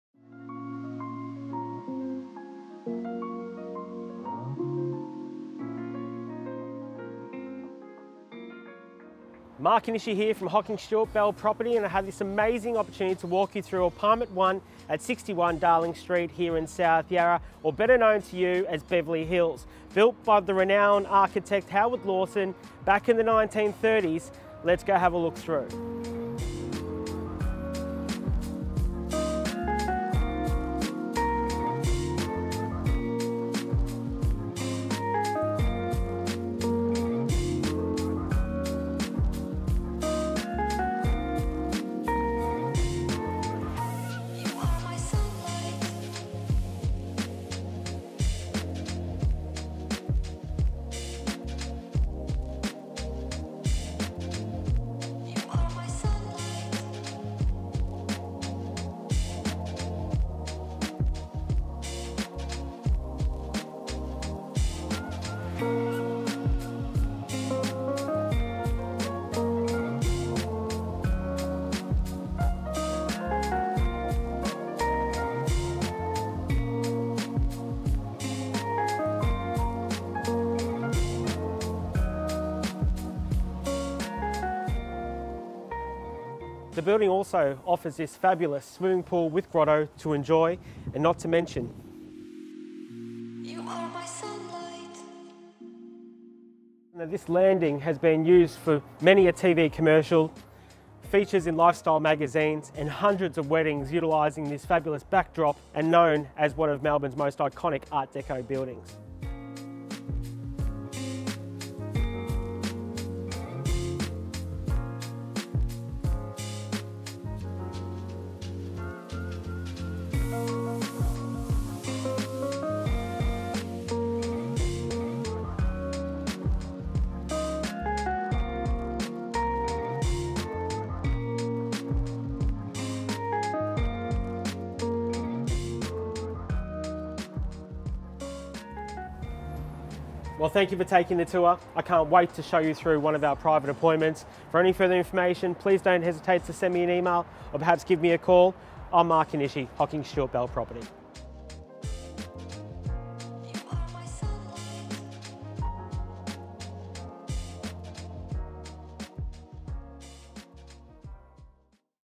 1/61 Darling St South Yarra
Enjoying a coveted location in sought after Beverley Hills, opposite Darling Gardens and the Yarra River, this north facing ground floor residence is quintessentially Art Deco with its superbly preserved period features and harmonious styling. Meticulously maintained, the interior comprises of an entrance lobby opening to the lounge (OFP), kitchen (DW), two bedrooms, sunroom/study, bathroom and laundry facilities. Includes heating, timber floors, alfresco area plus a residents pool.